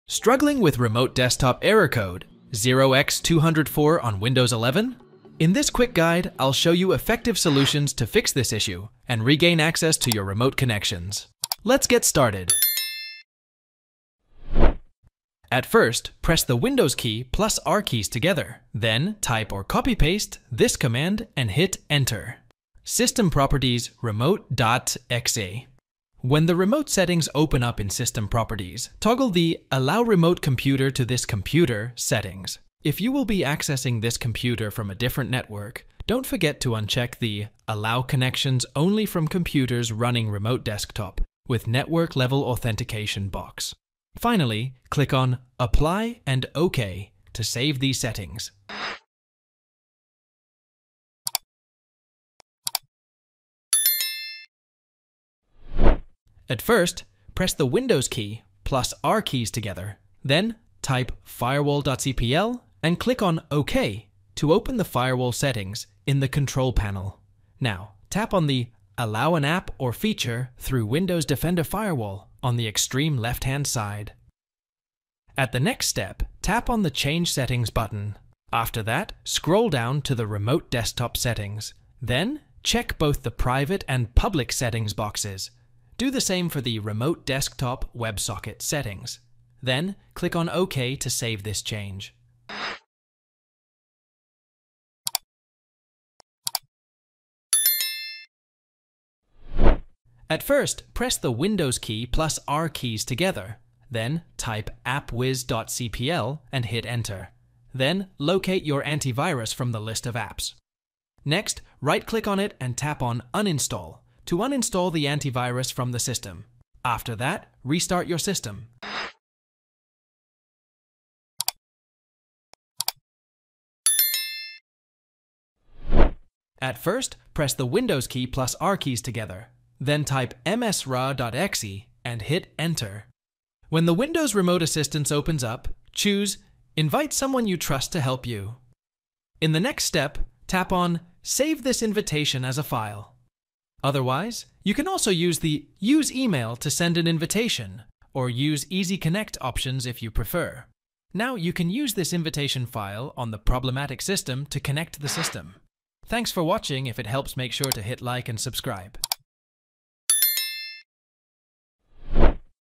Remote Desktop Error Code 0x204 on Windows 11-4 Fix
Encountering Remote Desktop Error Code 0x204 in Windows 11? This frustrating issue can prevent you from connecting to remote computers. Discover four effective solutions to troubleshoot and resolve this error, ensuring seamless remote access. We’ll guide you through the necessary steps to get you back online.

00:00 Intro
00:10 Allow Remote Computer Access
00:50 Adjust the firewall settings
01:35 Uninstall any third-party antivirus
02:00 Use the WRA invitation process
02:37 Outro